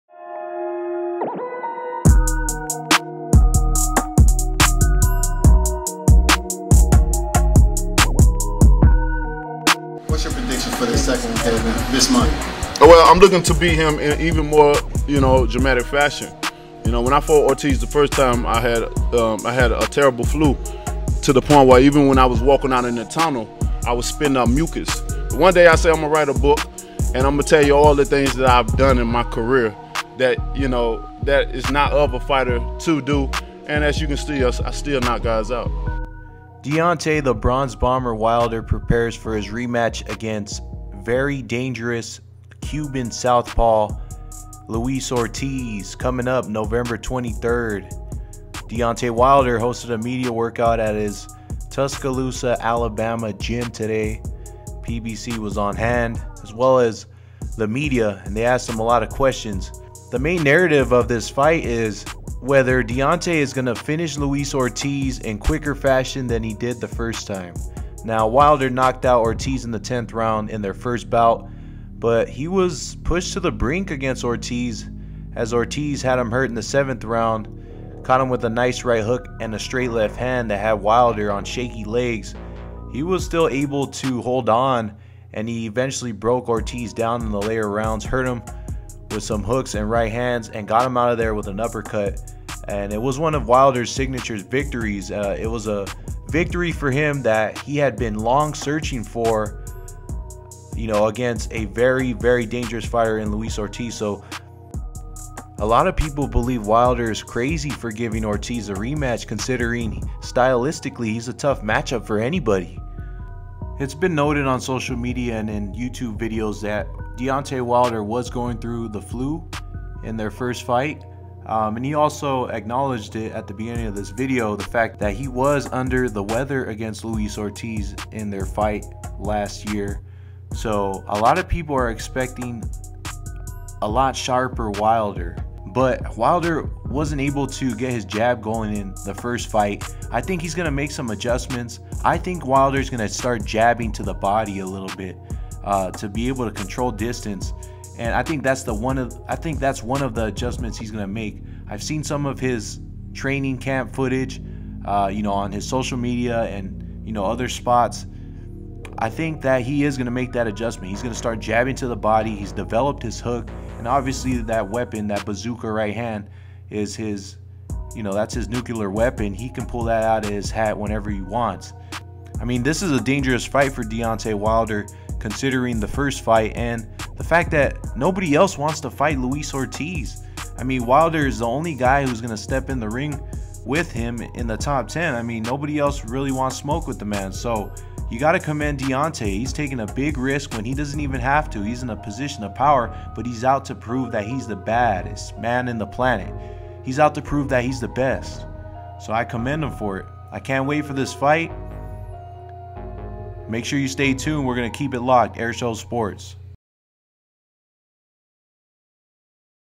DEONTAY WILDER Vows to Beat LUIS ORTIZ in MORE "DRAMATIC FASHION"
Deontay Wilder (41-0-1, 40 KOs) prepares for his rematch against dangerous southpaw Luis Ortiz (31-1, 26 KOs, 2 NC) November 23rd in Las Vegas, Nevada.